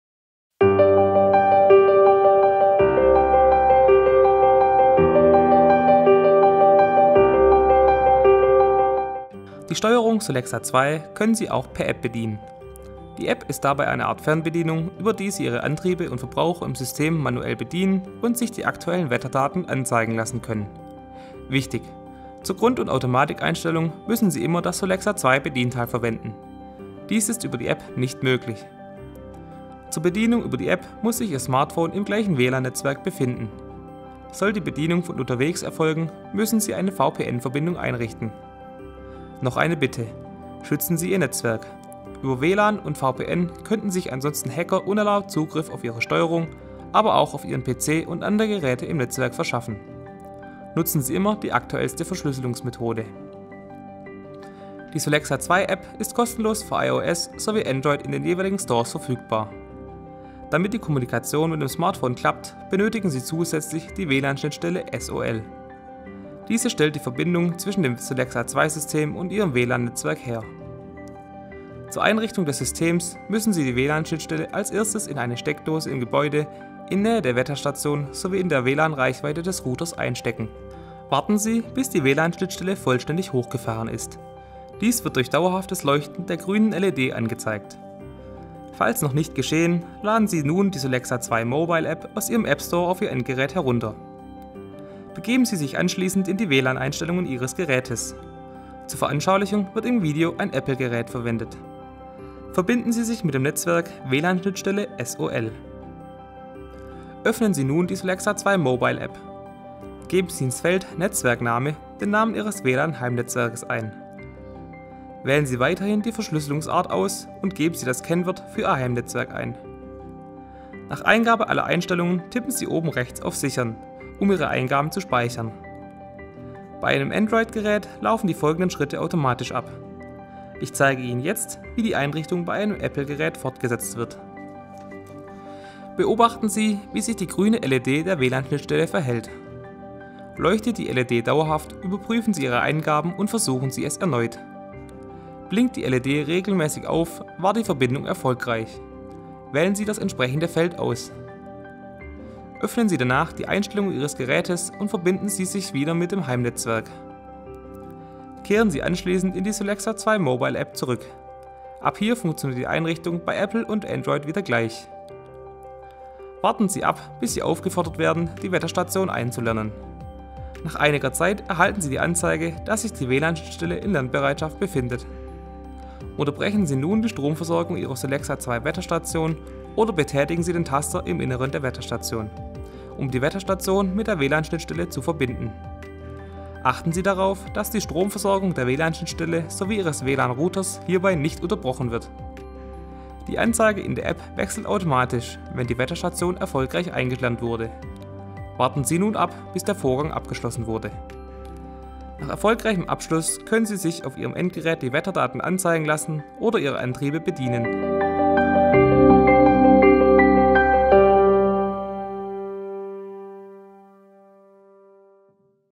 Solexa II – 12. App einrichten (WLAN-Schnittstelle SOL)
Antriebe und Geräte im Steuerungssystem Solexa II können auch per App bedient werden. Die Solexa II App ist kostenlos für iOS sowie Android in den jeweiligen Stores verfügbar. Damit die Kommunikation mit dem Smartphone klappt, benötigen Sie zusätzlich die WLAN Schnittstelle SOL. Diese stellt die Verbindung zwischen dem Solexa II System und Ihrem WLAN-Netzwerk her.

Nicht vergessen: Schützen Sie Ihr Netzwerk indem Sie immer die aktuellste Verschlüsselungsmethode für Ihr WLAN verwenden.

Untertitel auf Deutsch und Englisch verfügbar.